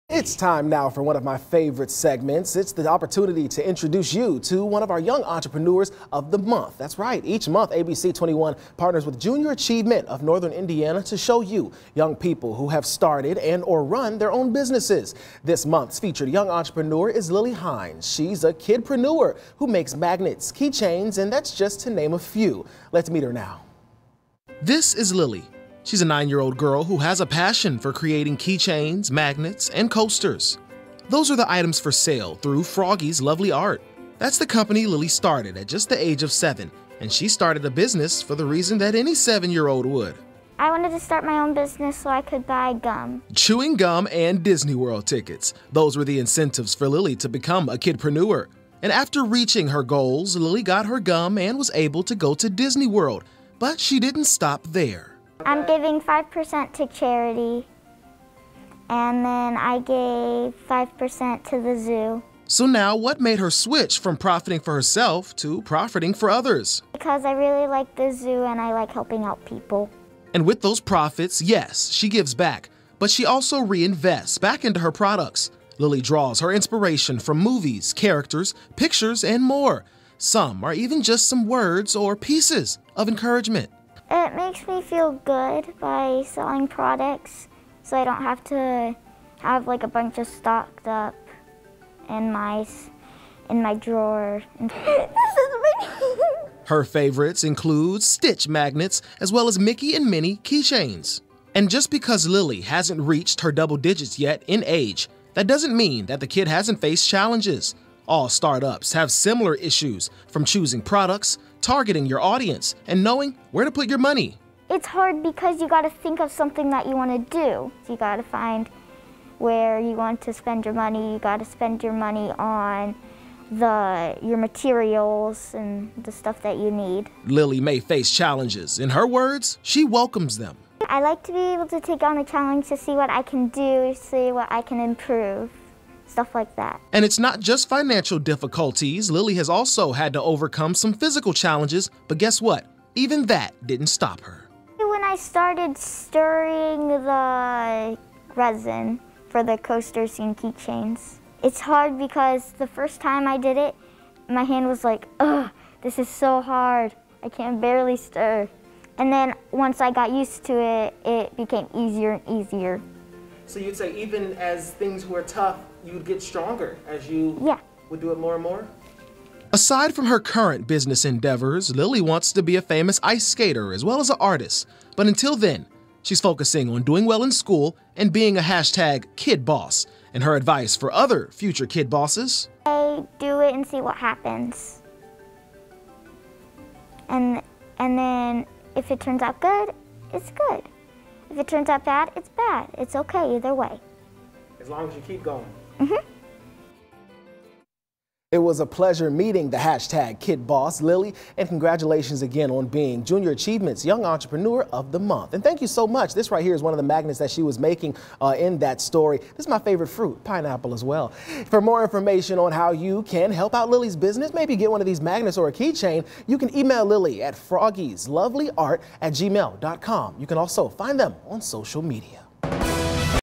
Junior Achievement’s Young Entrepreneur of the Month - Lily Hinds
We introduce to you to Lily Hinds. A 9-year-old girl who has a passion for creating key chains, magnets and coasters.